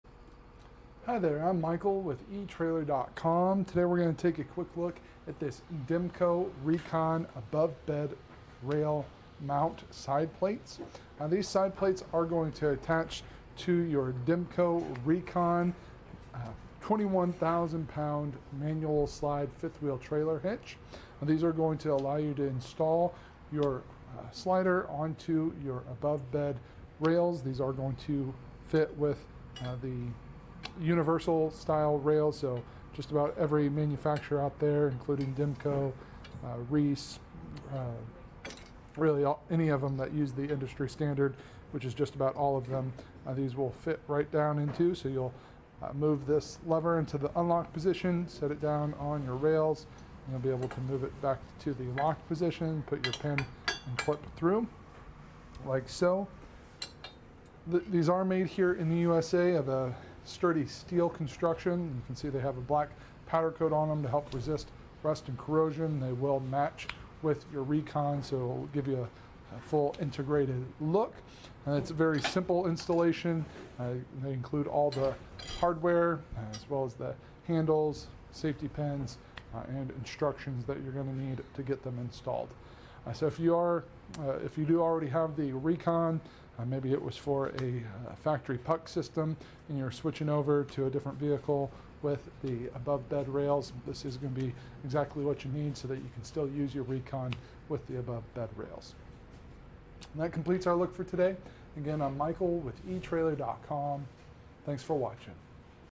etrailer | Comprehensive Review: Demco Recon Sliding 5th Wheel Hitch for Chevy/GMC
Click for more info and reviews of this Demco RV 5th Wheel Hitch:

Hi, there, I'm Michael with etrailer.com.  Today, we're gonna take a quick look at this Demco Recon above-bed rail mount side plates.  Now, these side plates are going to attach to your Demco Recon 21,000 pound manual slide fifth wheel trailer hitch and these are going to allow you to install your slider onto your above-bed rails.  These are going to fit with the universal-style rails, so just about every manufacturer out there including Demco, really any of 'em that use the industry standard, which is just about all of them, these will fit right down into.  So you'll move this lever into the unlock position, set it down on your rails, and you'll be able to move it back to the lock position.  Put your pin and flip through, like so.

These are made here in the USA of a sturdy steel construction.  You can see they have a black powder coat on 'em to help resist rust and corrosion.  They will match with your recon, so it'll give you a full integrated look.  And it's a very simple installation.  They include all the hardware as well as the handles, safety pins, and instructions that you're gonna need to get them installed.

So if you do already have the recon, maybe it was for a factory puck system and you're switching over to a different vehicle with the above-bed rails, this is gonna be exactly what you need so that you can still use your recon with the above-bed rails.  And that completes our look for today.